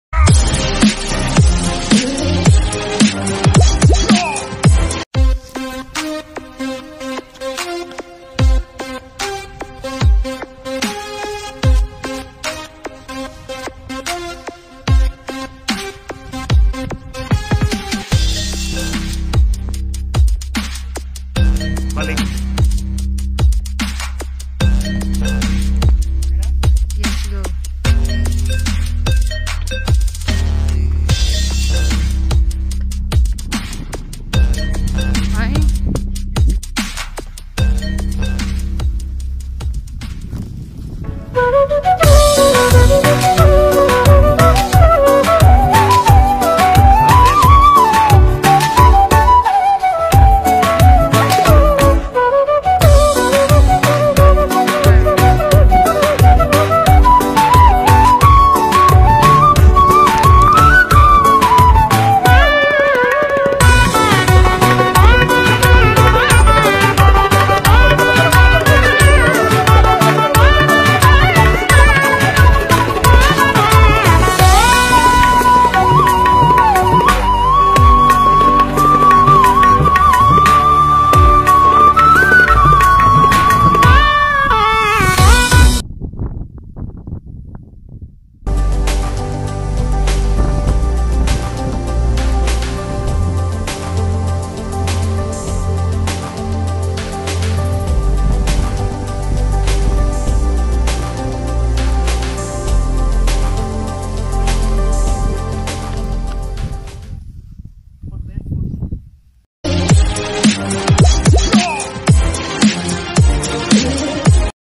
A Parkour with the Girlfriend
So I invited my girl to do a few basic parkour moves even though she's a complete novice. She did enjoy the whole thing.